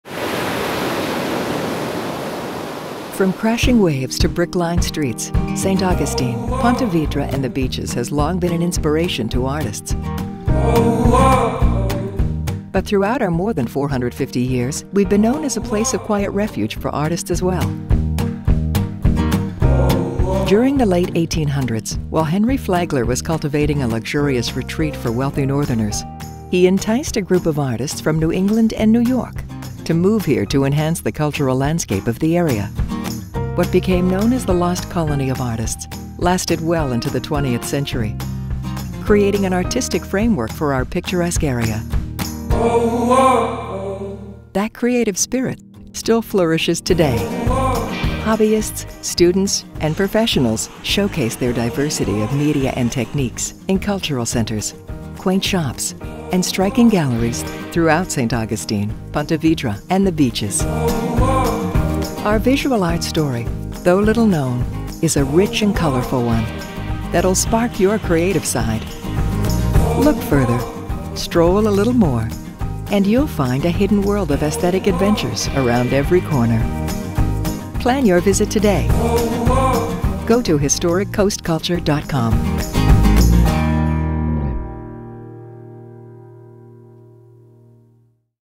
The Visual Arts of St. Augustine, Florida
From modern galleries and exclusive exhibits to the designs of its historic cathedrals, the Historic Coast’s visual arts offer an endless feast for the eyes – and the imagination.